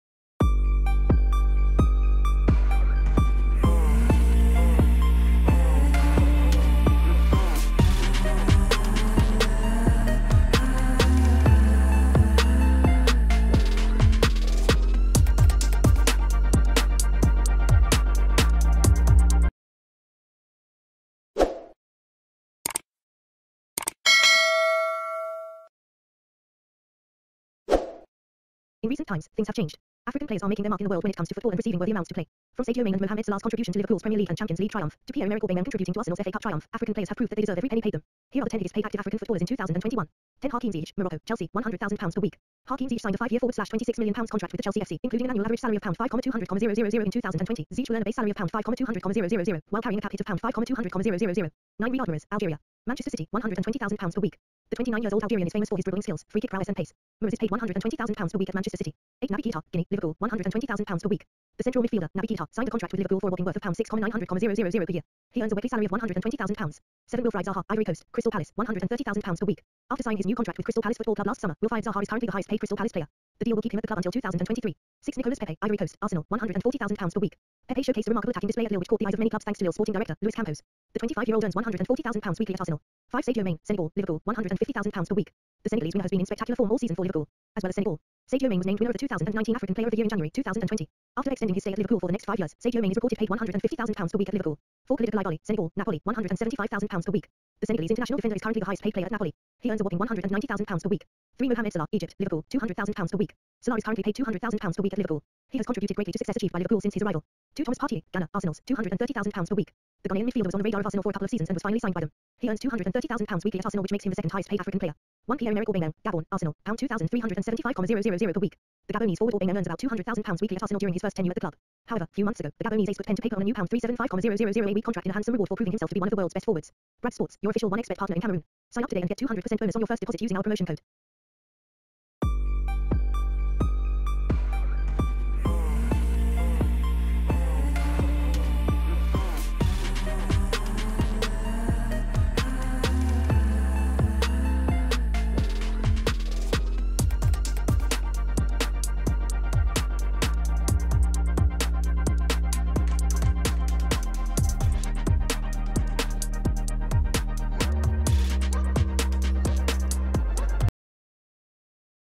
Top 10 highest paid African footballers in 2021
Top 10 Highest Paid African Footballers Right Now 2021 as Arsenal captain Aubameyang wins the day.
Years back African footballers were paid meagre amounts by their respective clubs.

In recent times, things have changed. African players are making their mark in the world when it comes to football and receiving worthy amounts to play.

From Sadio Mane and Mohammed Salah’s contribution to Liverpool’s Premier League and Champions League triumph, to Pierre-Emerick Aubameyang contributing to Arsenal’s FA cup triumph,  African players have proved that they deserve every penny paid them.

We, therefore look at the Top 10 highest paid Active African footballers in late 2021.